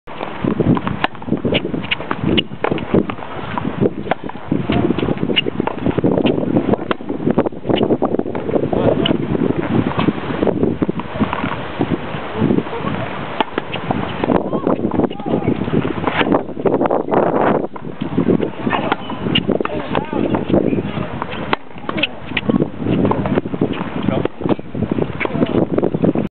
陳迪 -下盤動作 Chen Ti's FootWork #2
2009 Feb @ Maccabi's Tournament, Melbourne. Training with Yang Tsung-Hua